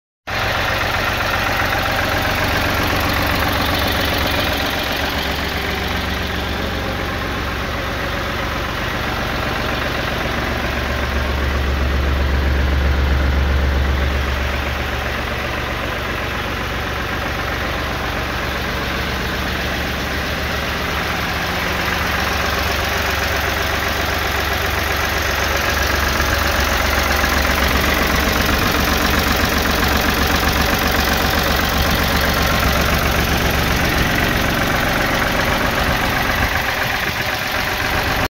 Ford 333
Lot 805 -Ford 333 be sold via Online Timed Auction ending Friday 24th February 2023 by Edward Buckland Ltd